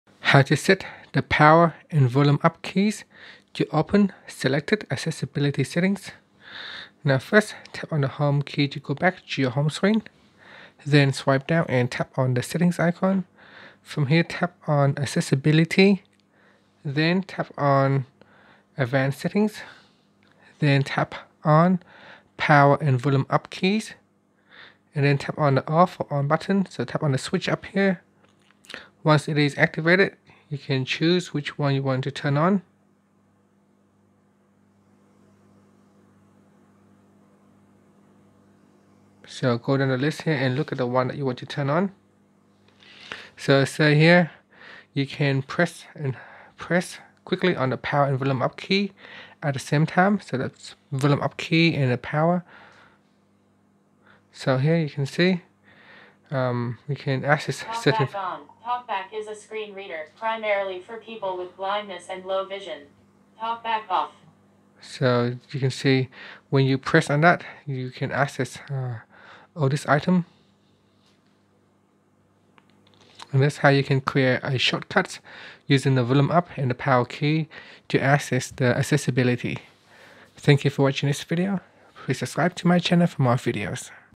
Galaxy S10 / S10+: Set Power + Volume Key To Open Selected Accessibility Settings
Learn how you can set Power + Volume key to open the selected accessibility Settings on Galaxy S10 / S10+.